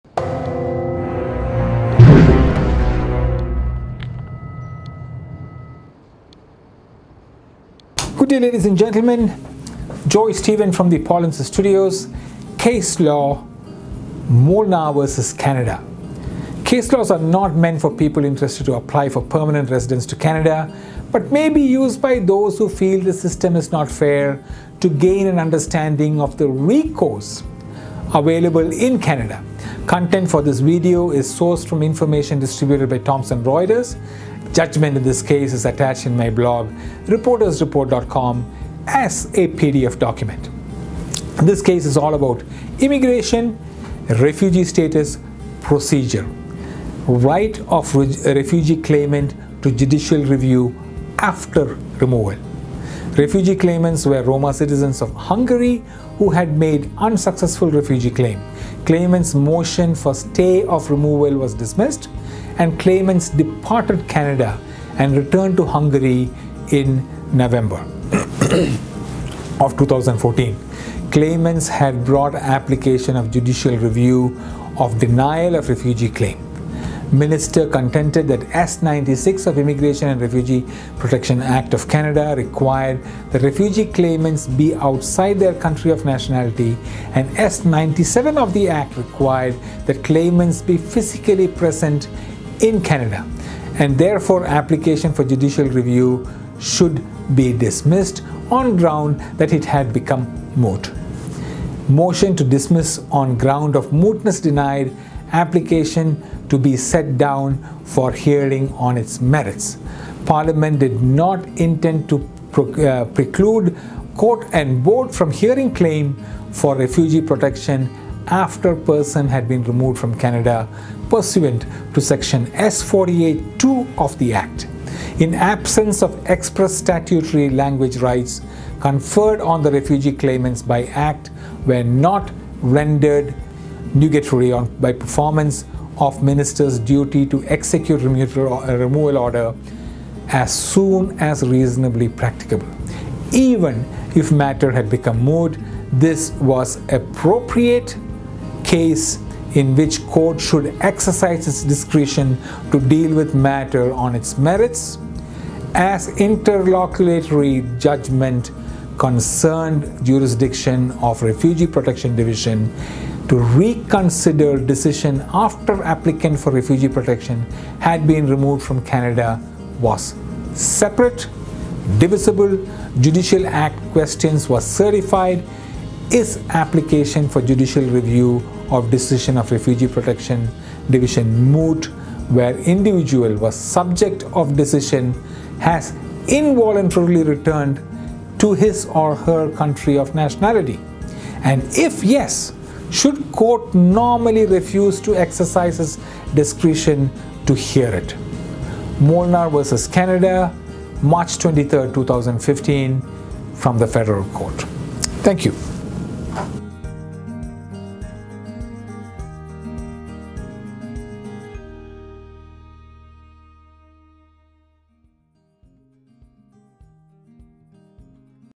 Canada Immigration Case-law Canada v Molnar IMMIGRATION — Refugee status — Procedure
IMMIGRATION — Refugee status — Procedure

Right of refugee claimant to judicial review after removal — Refugee claimants were Roma citizens of Hungary who had made unsuccessful refugee claim — Claimants' motion for stay of removal was dismissed and claimants departed Canada and returned to Hungary in November 2014 — Claimants had brought application for judicial review of denial of refugee claim — Minister contended that s. 96 of Immigration and Refugee Protection Act (Can.), required that refugee claimants be outside their country of nationality, and s. 97 of Act required that claimants be physically present in Canada, and therefore application for judicial review should be dismissed on ground that it had become moot — Motion to dismiss on ground of mootness denied; application to be set down for hearing on its merits — Parliament did not intend to preclude court and board from hearing claim for refugee protection after person had been removed from Canada pursuant to s. 48(2) of Act — In absence of express statutory language rights conferred on refugee claimants by Act were not rendered nugatory by performance of Minister's duty to execute removal order as soon as reasonably practicable — Even if matter had become moot, this was appropriate case in which court should exercise its discretion to deal with matter on its merits — As interlocutory judgment concerned jurisdiction of Refugee Protection Division to reconsider decision after applicant for refugee protection has been removed from Canada was separate, divisible, judicial act, question was certified: Is application for judicial review of decision of Refugee Protection Division moot where individual who was subject of decision has involuntarily returned to his or her country of nationality, and, if yes, should court normally refuse to exercise its discretion to hear it?